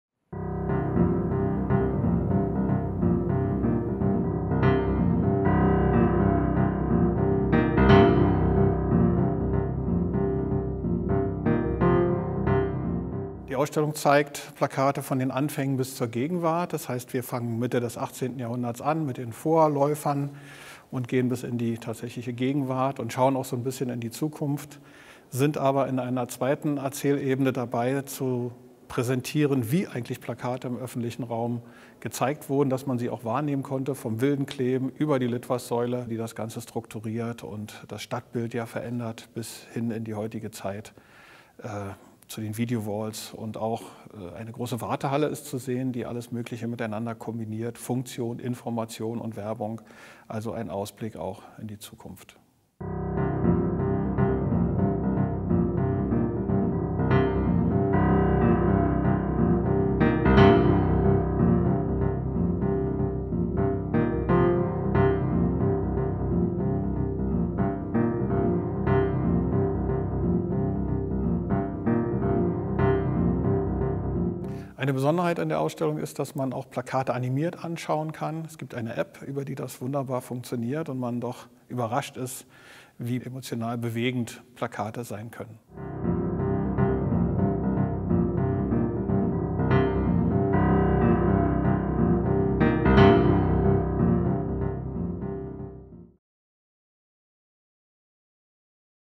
"WE WANT YOU!" Trailer
Unabhängig von der Stil-Geschichte des Plakats ist die Art und Weise, wie Plakate im öffentlichen Raum präsentiert werden, entscheidend für ihre Wahrnehmung und Wirkung. Beginnend mit dem „wilden Kleben“ etabliert sich Mitte des 19. Jahrhunderts die Werbung an den Litfaßsäulen. Ab den 1920er Jahren entstehen Kampagnen und bis zur Einführung des Fernsehens Mitte der 1950er Jahre bleibt das Plakat das Leitmedium der Werbung. Mit der Einführung der Großfläche gewinnt es eine neue starke Sichtbarkeit. Fortan bestimmen technische Neuerungen den Wandel hin zu neuen Formaten und permanenter Präsenz. Durch Digitalisierung und Vernetzung, durch interaktive Elemente mit direkter Ansprache und zielgruppenorientierter Aussteuerung wird der öffentliche Auftritt des Plakats erweitert. Von den ersten Erscheinungsformen des Plakats bis in seine digitale Gegenwart und Zukunft spannt die Ausstellung anhand von wichtigen Exponaten der Plakatgeschichte, u. a. von Lucian Bernhard, Isolde Baumgart, Hans Hillmann, Alfons Maria Mucha, Henri de Toulouse-Lautrec und Charles Paul Wilp, einen kultur- und medienhistorischen Bogen.

8. Apr 2022 – 28. Aug 2022
WE WANT YOU!
Von den Anfängen des Plakats bis heute

Quite apart from the stylistic history of the poster, the way in which posters are presented in public space has played a decisive part in their public perception and impact. Beginning “wild flyposting”, in the mid-19th century advertising started to be channelled through ad stelae. From the 1920s onwards, campaigns started to be crafted. Until the introduction of television in the mid-1950s, posters remained the leading advertising medium. With the introduction of large-format billboards, posters gained a new, emphatic presence. From then on, technological innovations drove the change to new formats and a permanent presence. Digitization and networking, interactive elements that address the viewer directly and target-group-based calibration all serve to expand posters’ public image.

8. Apr 2022 – 28. Aug 2022
WE WANT YOU!
From the Beginnings of the Poster until Today